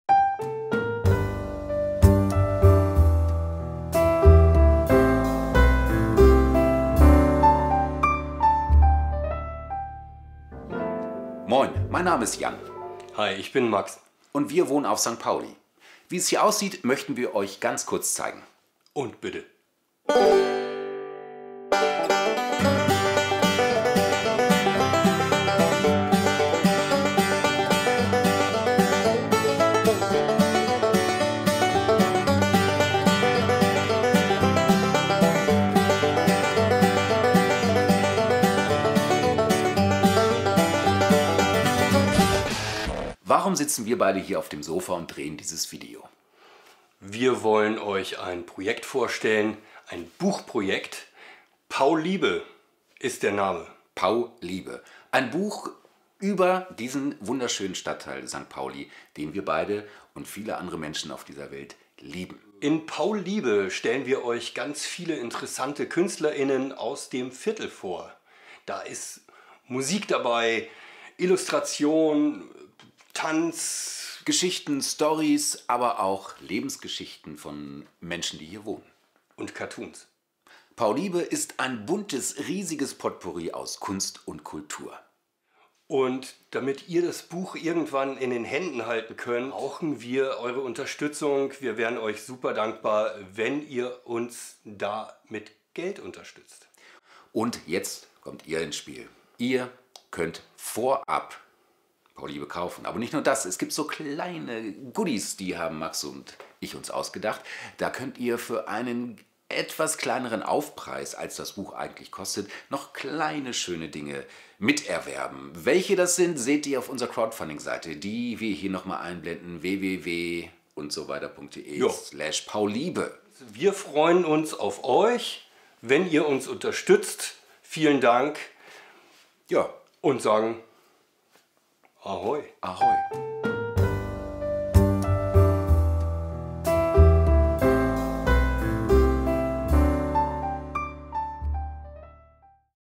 PauliebeCrowd
Endlich! Das langerwartete ultimative St. Pauli Buch entsteht. Wenn ihr uns mithilfe von Crowdfunding unterstützt, kann die Druckerei loslegen.